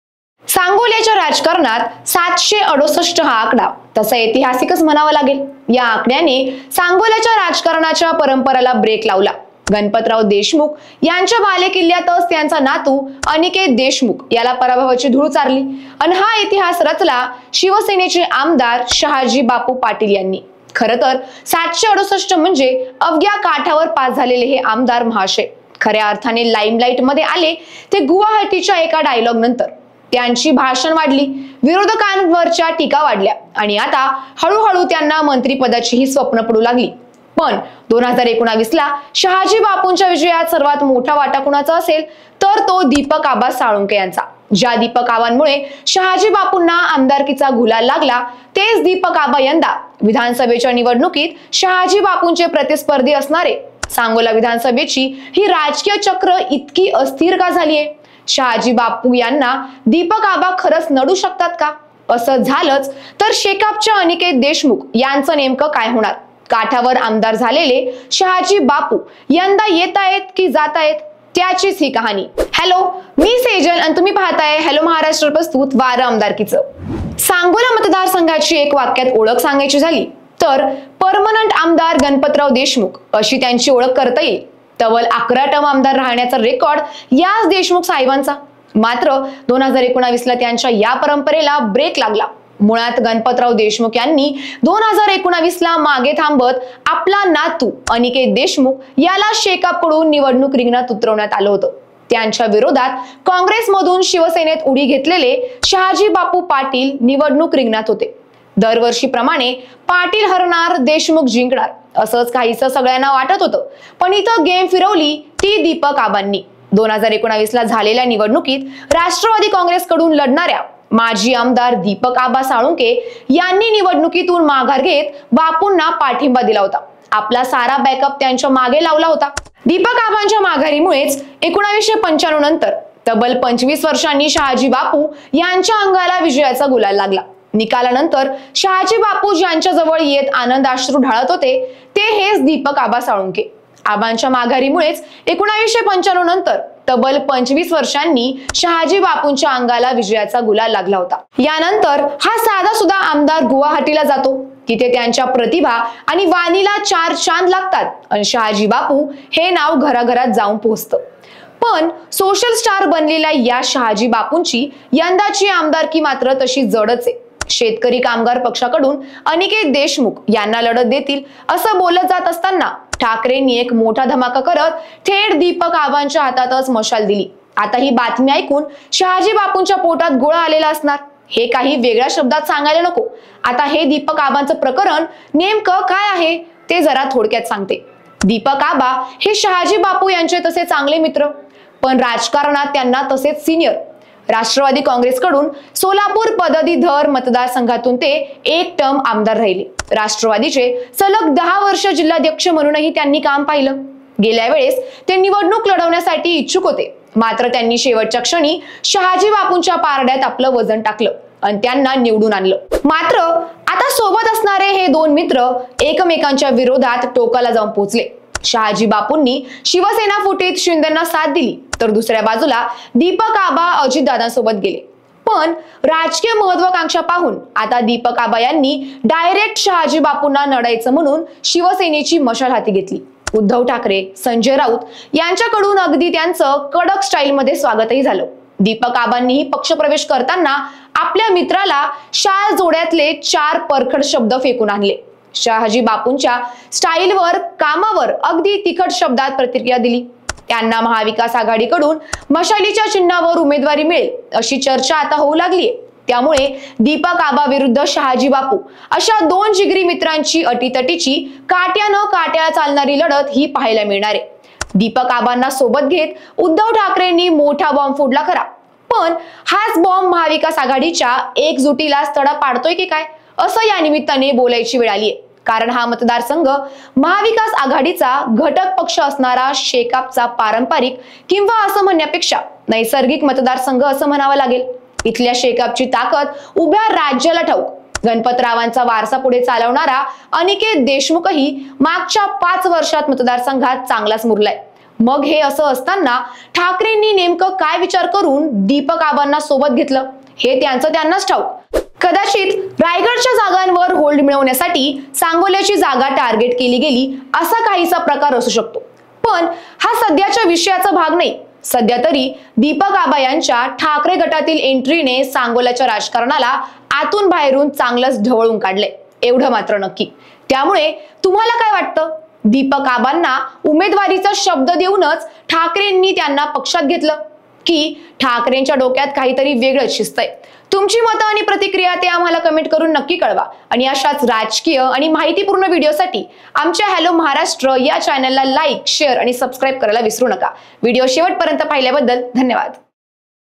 Dipak Aba Salunkhe यंदा Sangola Vidhan Sabha मतदारसंघाचा निकाल फिरवणार | Shahaji Bapu Patil
सांगोल्याच्या राजकारणात 768 हा आकडा तसा ऐतिहासिकच म्हणावा लागेल... या आकड्याने सांगोल्याच्या राजकारणाच्या परंपरेला ब्रेक लावला... गणपतराव देशमुख यांच्या बालेकिल्ल्यातच त्यांचा नातू अनिकेत देशमुख याला पराभवाची धूळ चारली... अन् हा इतिहास रचला शिवसेनेचे आमदार शहाजी बापू पाटील... विरोधकांवरच्या टीका वाढल्या... आणि आता हळूहळू त्यांना मंत्रिपदाचीही स्वप्न पडू लागली... पण 2019 ला शहाजीबापूंच्या विजयात सर्वात मोठा वाटा कुणाचा असेल तर तो दीपक आबा साळुंखे यांचा... ज्या दीपक आबांमुळे शहाजी बापूंना आमदारकीचा गुलाल लागला तेच दीपक आबा यंदा विधानसभेच्या निवडणुकीत शहाजी बापूंचे प्रतिस्पर्धी असणार आहेत... सांगोला विधानसभेची ही राजकीय चक्र इतकी अस्थिर का झालीयेत? शहाजीबापू यांना दीपक आबा खरच नडू शकतात का? असं झालंच तर शेकापच्या अनिकेत देशमुख यांचं नेमकं काय होणार? काठावर आमदार झालेले शहाजी बापू यंदा येतायेत की जातायत? त्याचीच ही कहाणी...

The number 768 holds a significant place in Sangola’s political history—it marked the end of a longstanding tradition. In Ganpatrao Deshmukh’s stronghold, his grandson Aniket Deshmukh faced a surprising defeat, and Shiv Sena MLA Shahaji Bapu Patil emerged victorious, rewriting history. Criticism followed, but Shahaji Bapu began to dream bigger, even of a ministerial position. However, the key figure behind his 2019 victory was Deepak Aba Salunkhe, who now, in a twist of fate, is gearing up to contest against Shahaji Bapu in this year's election. The political cycle in Sangola has become highly unstable—will Deepak Aba's challenge topple Shahaji Bapu? And what about Shekap's Aniket Deshmukh? Is Shahaji Bapu's reign nearing its end, or will he secure another term? This is the evolving story of Sangola politics.